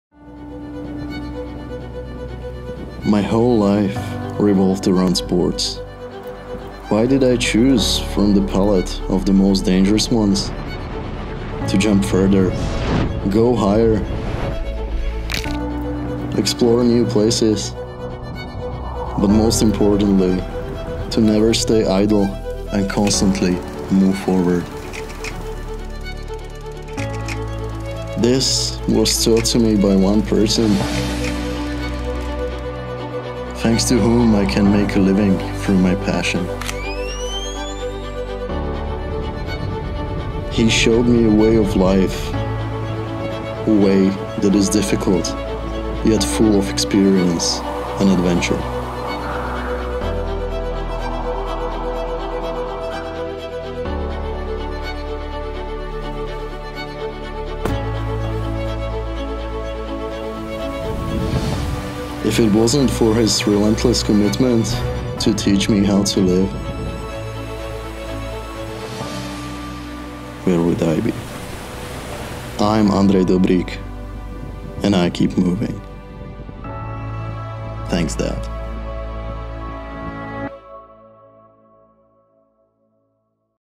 Make Your Move: With Andrej Dobrík and the V class
I always wanted to tell story about my dad and how he played the major role in my life. Thanks to him I am where I am and I couldn´t be more grateful for all that he and my mom did for me.

I got the chance to work with Slovak Mercedes-Benz dealership in Nitra city and got inspired by stories that are made by official the Mercedes-Benz called Make Your Move. So I wanted to adapt this style of film making.

This is not an official commercial for Mercedes-Benz and they have not commissioned me to do this video.